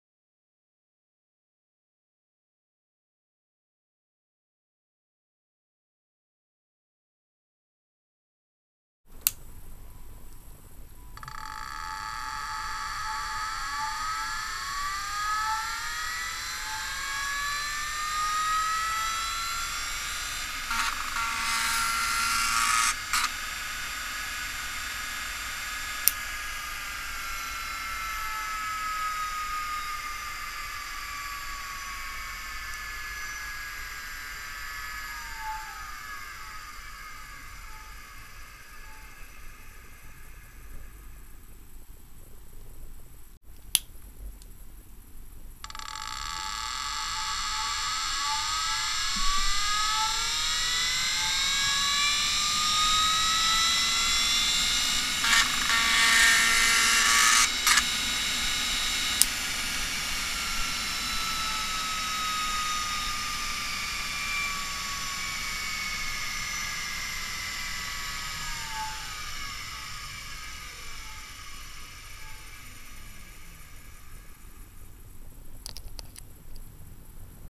Old Hard Drive
Old MiniScribe hard drive model 3650.
Specs:
5.25 inch form factor
MFM connector
40MB capacity